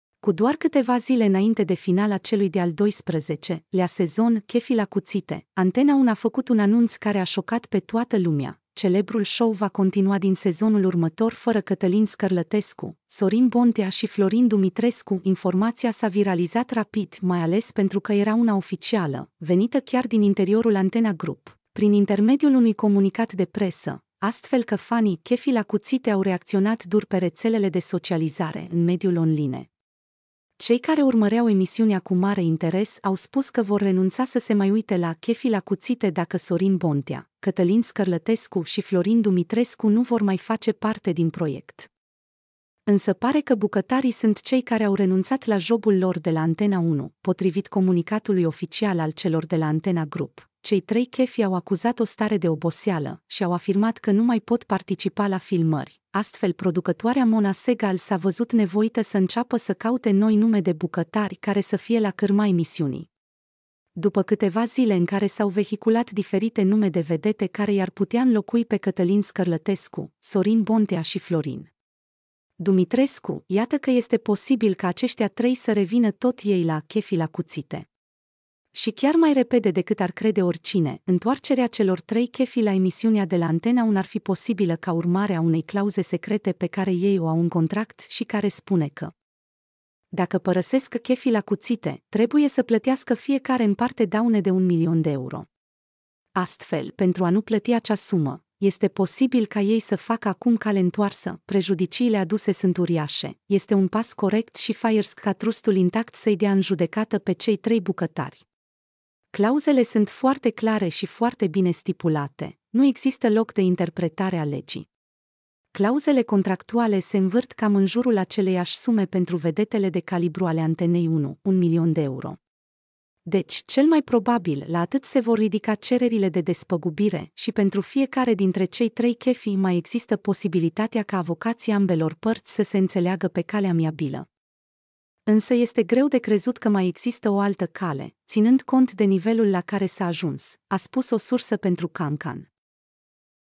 Deja se întorc Bontea, Scărlătescu și Dumitrescu la Chefi la Cuțite ?
Prejudiciile aduse sunt uriașe, este un pas corect și firesc ca trustul Intact să-i dea în judecată pe cei trei bucătari. Clauzele sunt foarte clare și foarte bine stipulate, nu există loc de interpretare a legii. Clauzele contractuale se învârt cam în jurul aceleiași sume pentru vedetele de calibru ale Antenei 1: un milion de euro. Deci, cel mai probabil, la atât se vor ridica cererile de despăgubire și pentru fiecare dintre cei trei chefi.